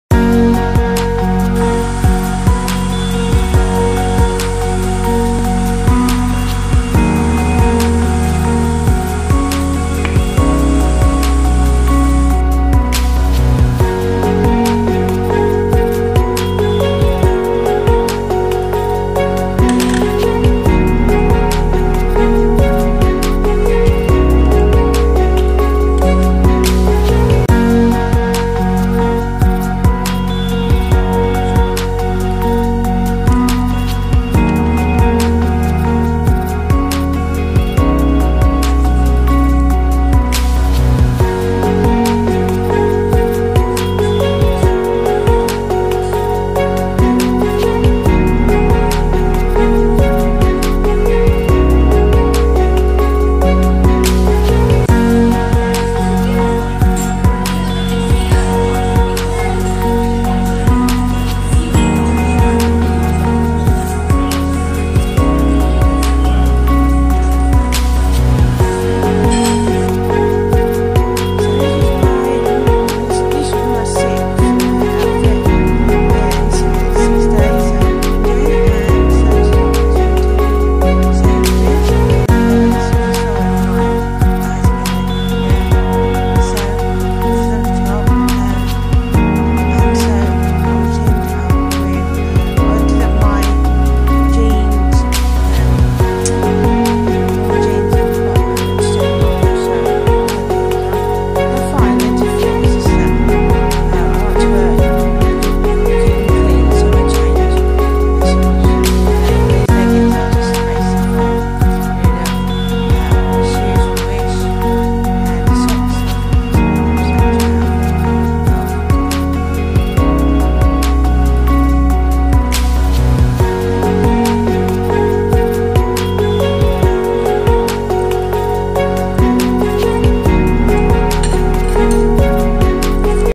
Saturday DIAL vid/working on books and blogs/child free weekend
New video, Day in a life/snippet of a fashion vid I'm putting together. My son was with his dad so free day. I like to chill and plan more projects to come